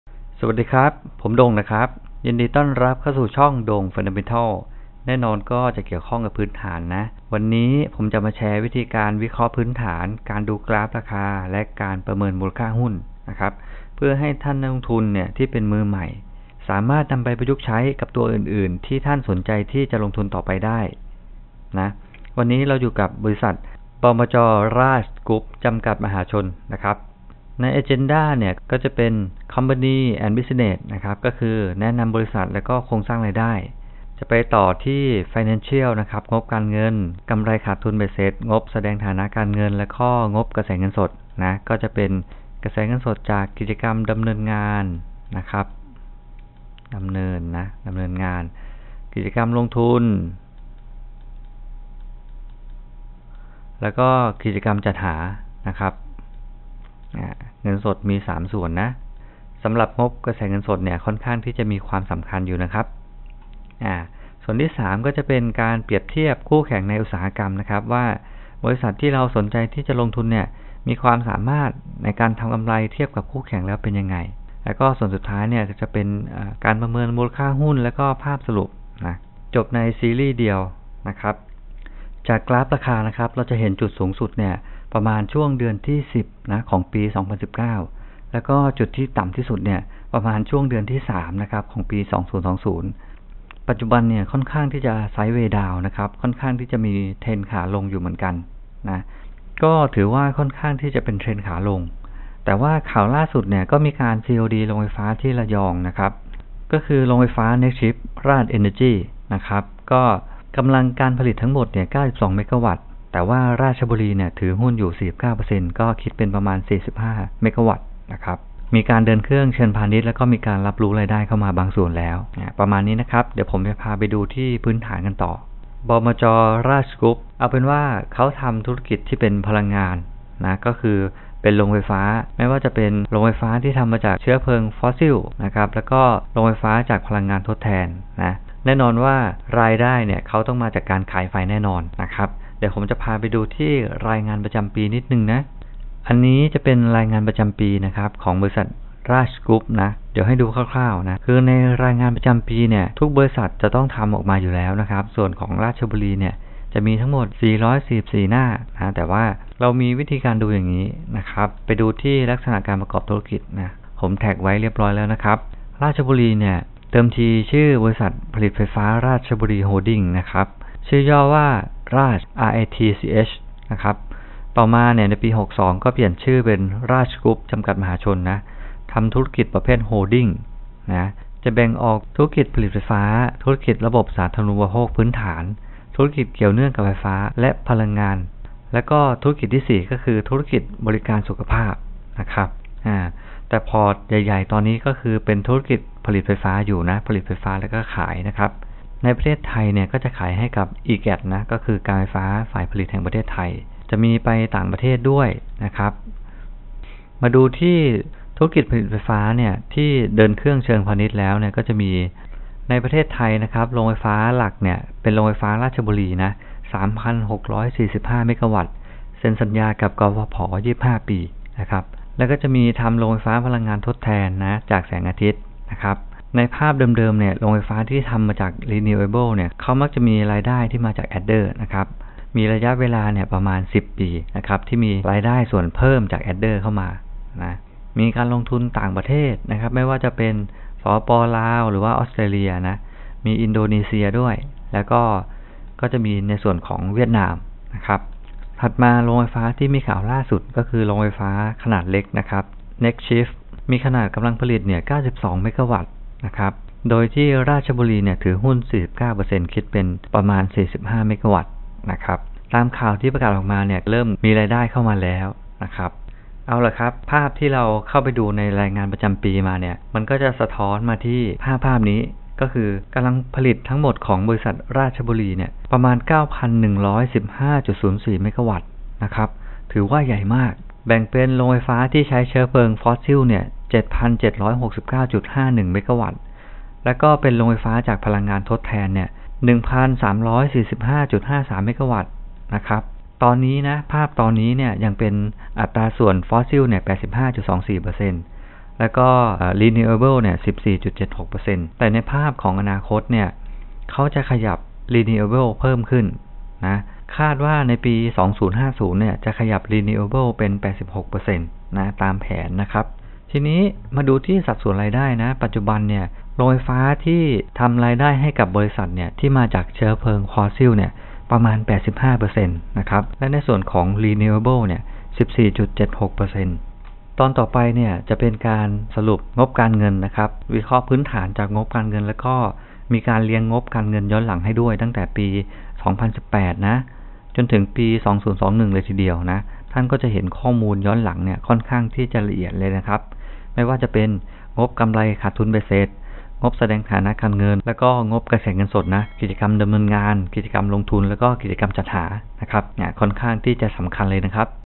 หุ้น RATCH วิเคราะห์พื้นฐาน อ่านกราฟราคา และประเมินมูลค่าหุ้น [ลงทุนพื้นฐาน] ตอนที่ 1 | EP - 47
** ปรับสปีด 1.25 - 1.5x เพื่อให้กระชับ **

** ส่ง Email มาขอไฟล์ งบการเงิน และไฟล์ Presentation PowerPoint นำเสนอ เพื่อสามารถนำไปวิเคราะห์ต่อได้ ด้วยตนเองครับ
ส่งมาขอได้ที่ Email หรือ Inbox Message ใน Facebook เพจได้เลยนะครับ **

CH-1 : Intro, แนะนำบริษัท และโครงสร้างรายได้ของบริษัท

CH-2 : Financial งบการเงิน (กำไร, ฐานะ, กระแสเงินสด)

[ พร้อมงบการเงินย้อนหลังในอดีต ฉบับเต็มปี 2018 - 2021]

ช่องทางการรับชม // ฝากติดตามทีนะครับ